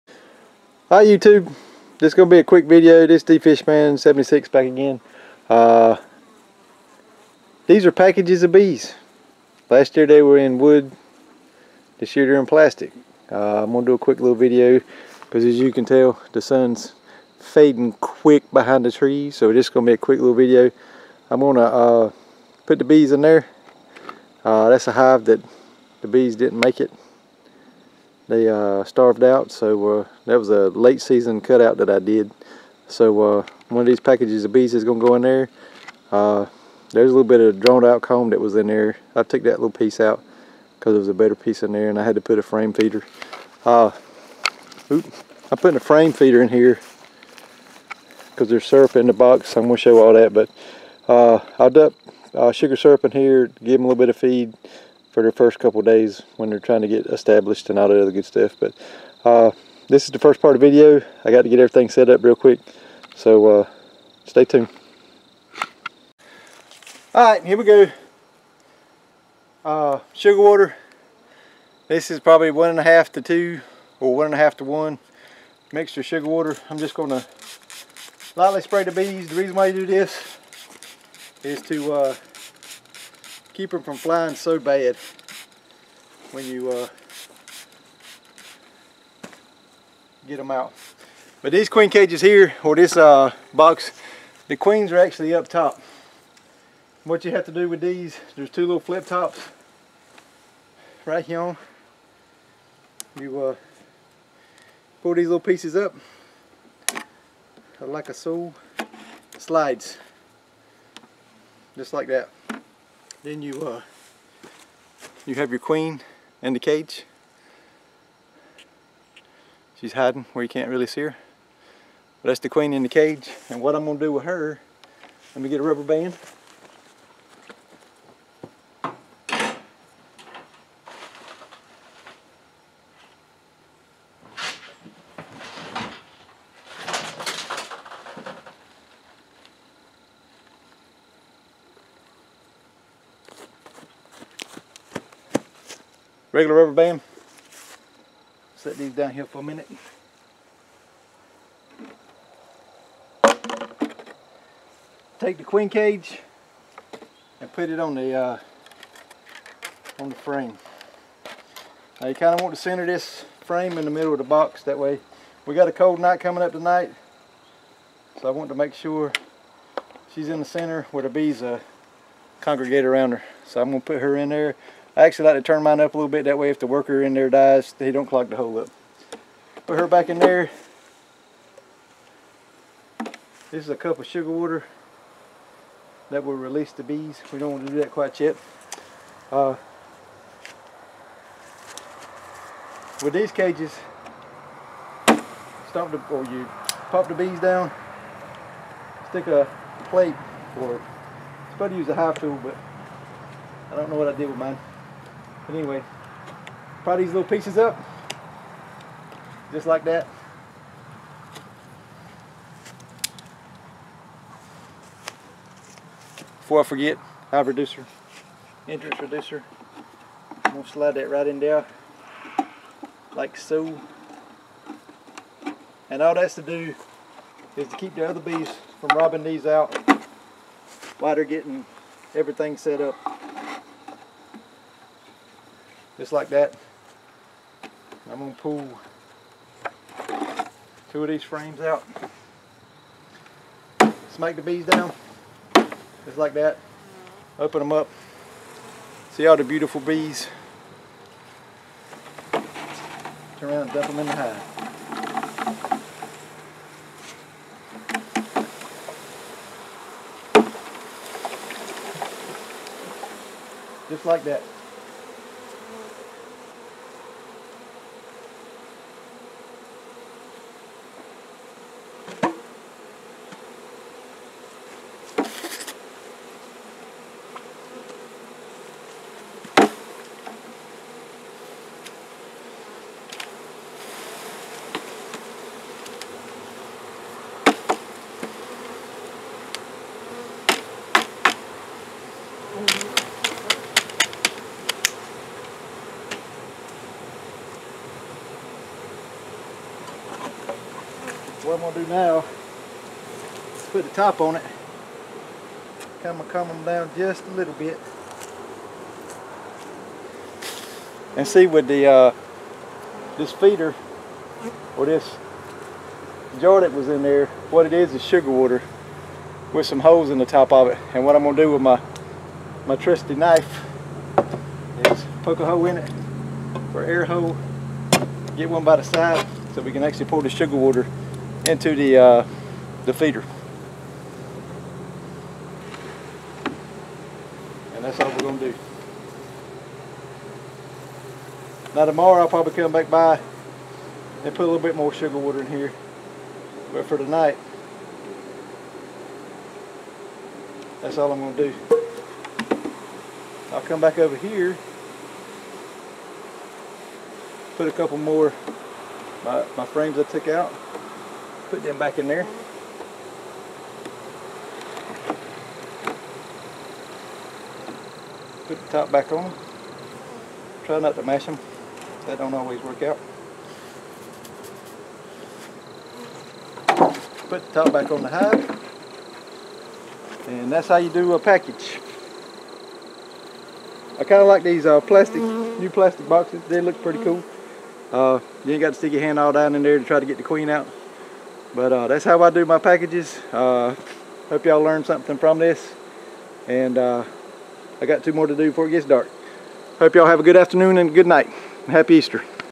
Honeybee package installation from the Bee Bus
Honey extractor I have installing honeybee packages